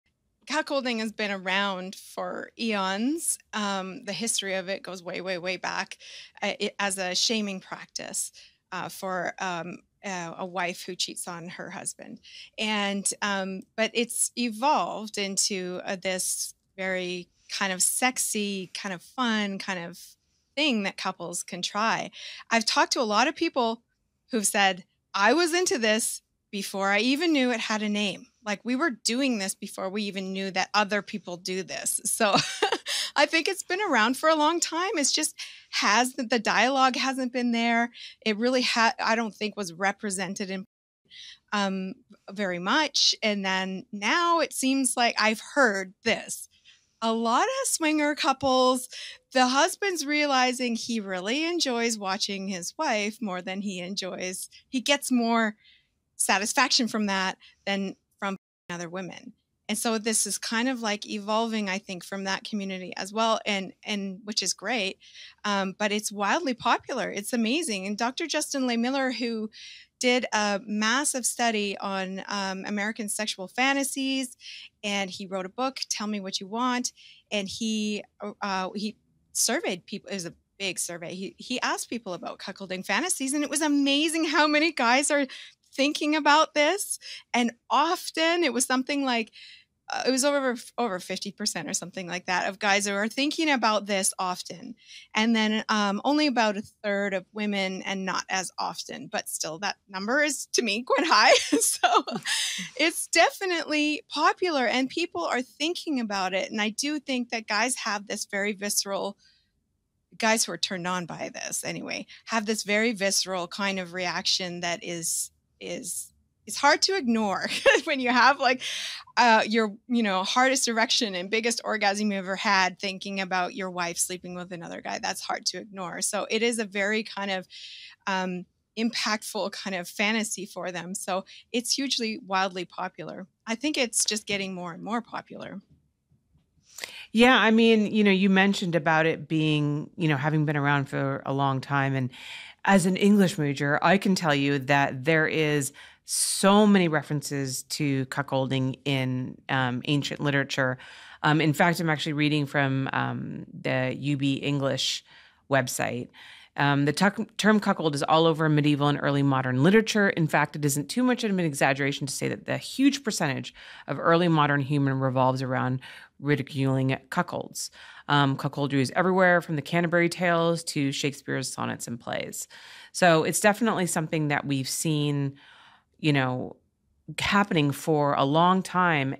Venus Cuckoldress: Cuckolding Has Been Around For Centuries
Just because it’s popular now doesn’t mean cuckolding is a new phenomenon!